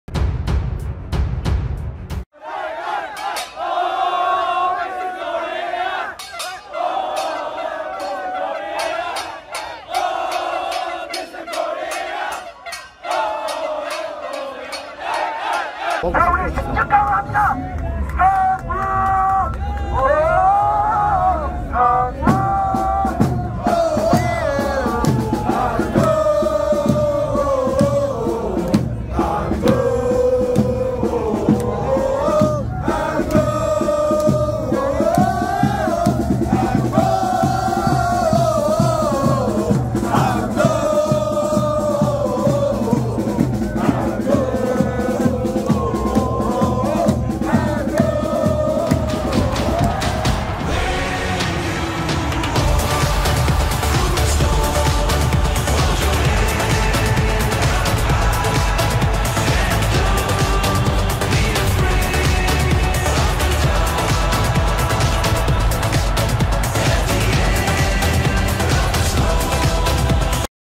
SUL-COREANOS COMEMORANDO A CLASSIFICAÇÃO PARA AS OITAVAS EM CIMA DE PORTUGAL NA COPA DO CATAR
Torcida da Coreia do Sul no Qatar, comemorando a classificação para as oitavas de finais da Copa do Mundo 2022.

00:00 : INTRODUÇÃO!
00:09 : INSCREVA-SE NO CANAL!
01:00 : DEIXE SEU LIKE!